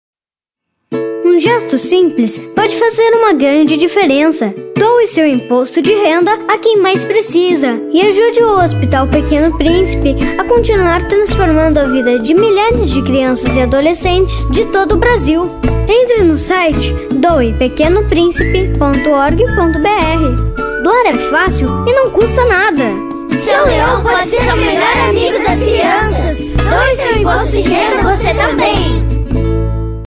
Campanha Doação de Imposto de Renda | HOSPITAL PEQUENO PRÍNCIPE
Doar seu imposto de renda é um gesto simples que pode fazer toda a diferença!
Ajude o Hospital Pequeno Príncipe a transformar a vida de milhares de crianças e adolescentes.